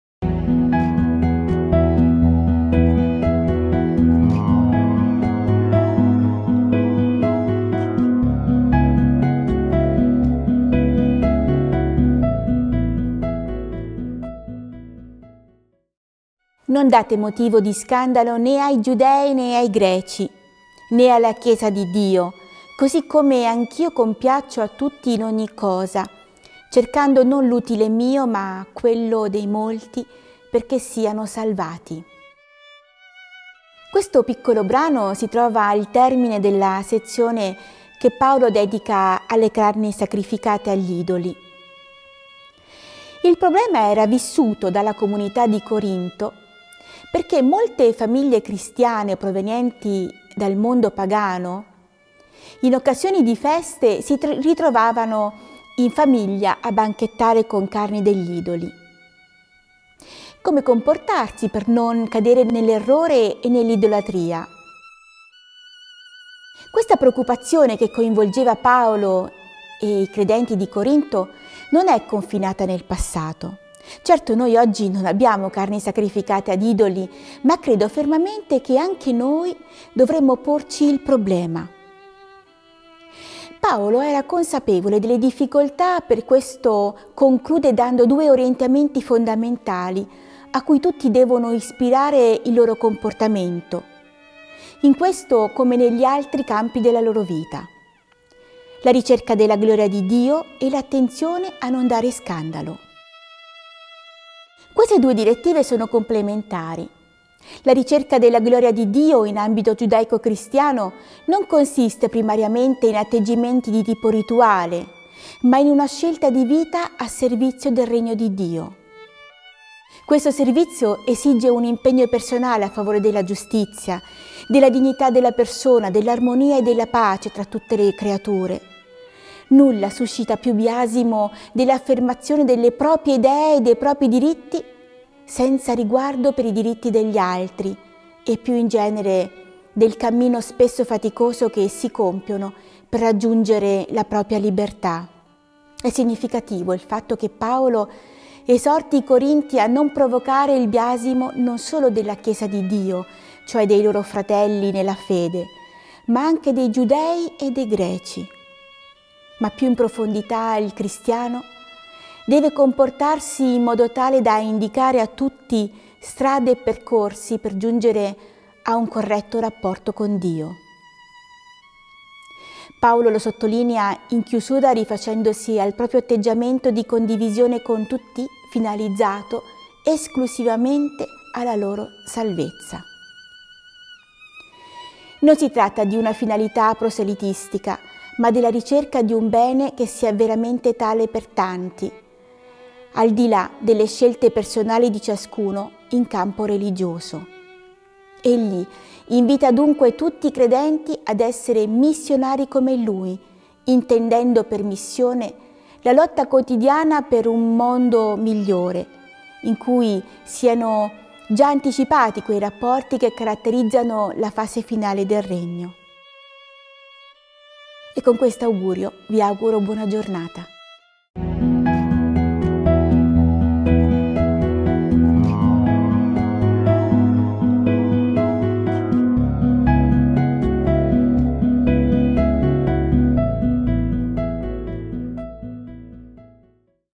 5 Febbraio 2017
“Non date motivo di scandalo né ai Giudei, né ai Greci, né alla chiesa di Dio; così come anch'io compiaccio a ...
Testi e presentazione in studio a cura di Laura Paris.